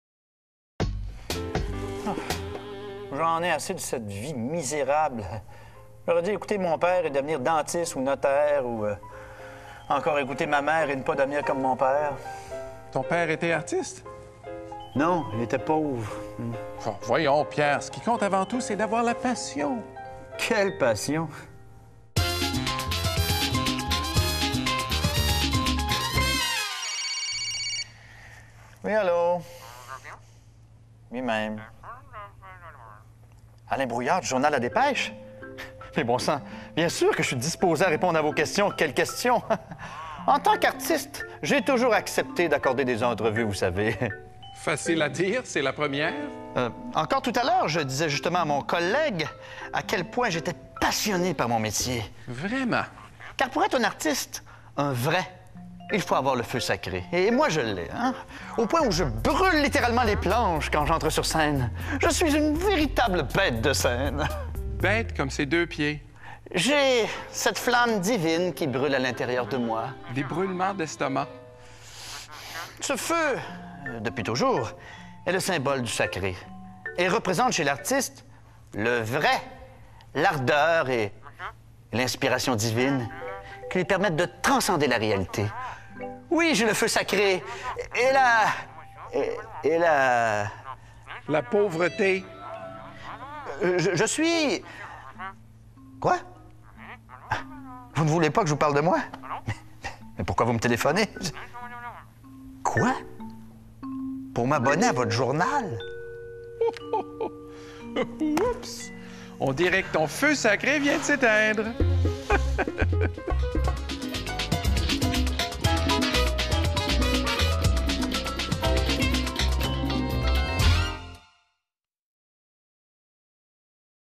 French Expressions Ep.11 Demo Reel
Through funny short stories, we learn the meaning and origins of many French expressions.